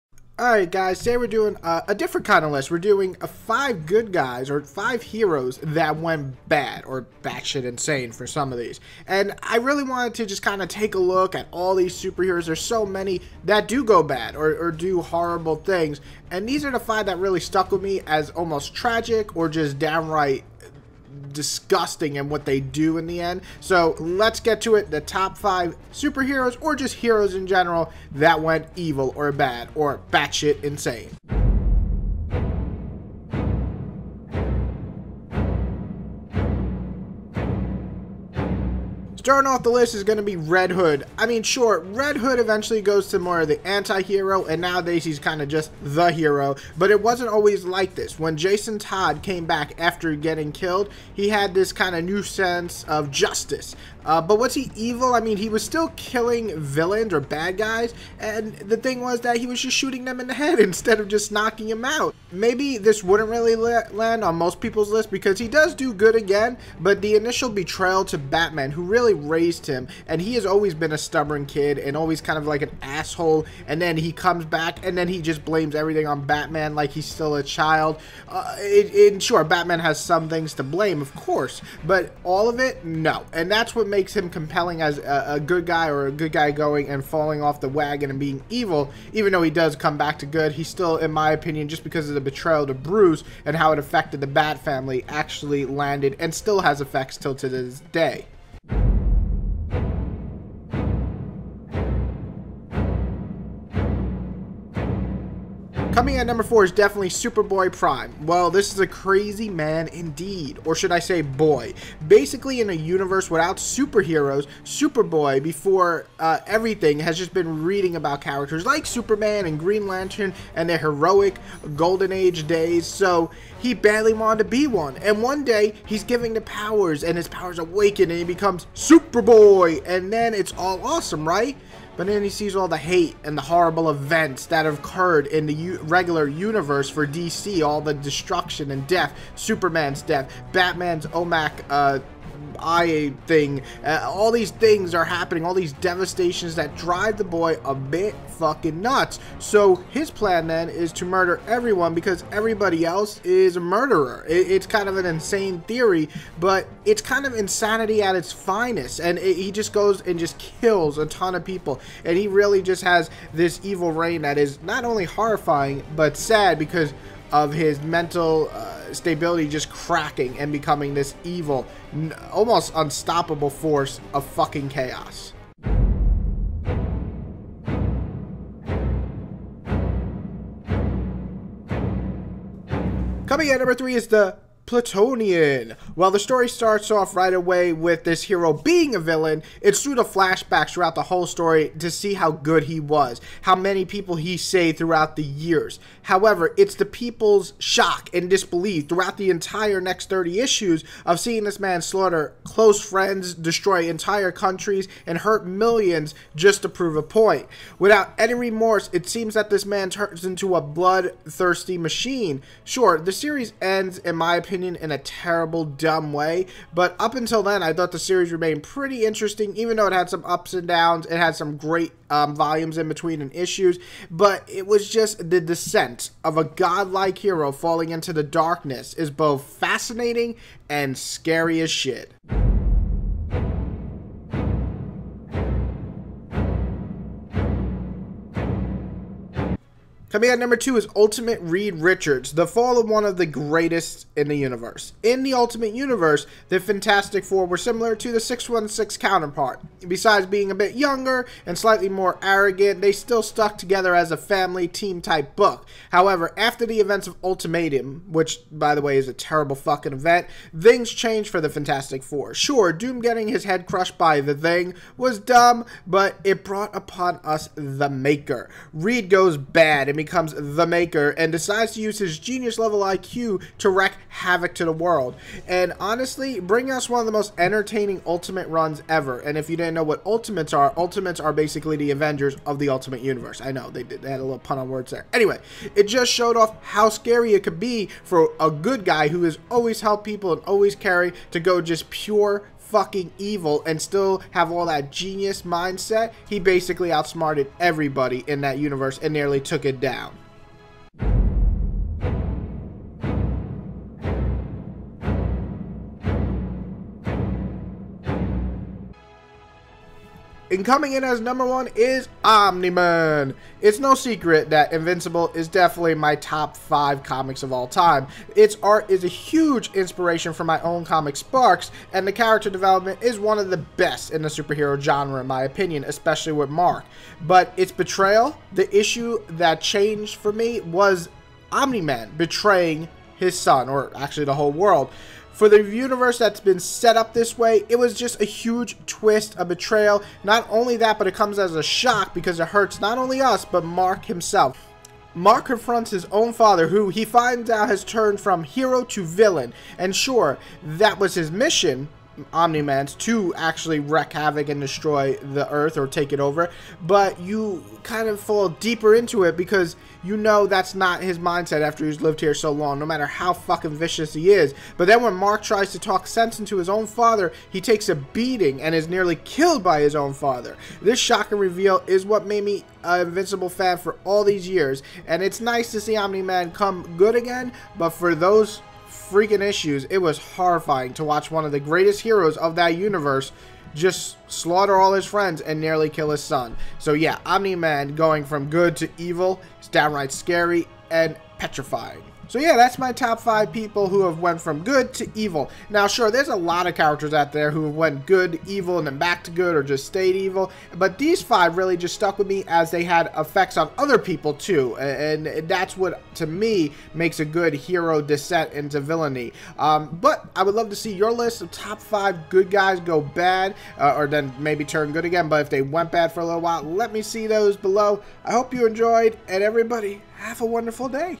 Top 5 Heroes Who Become Villains In Comics
So there's a ton of heroes in the world of comics from DC/Marvel/IMAGE/BOOM and more. However, what happens when that good guy goes evil? How does it effect the other heroes? We talk about 5 who betrayed their ways and hurt others!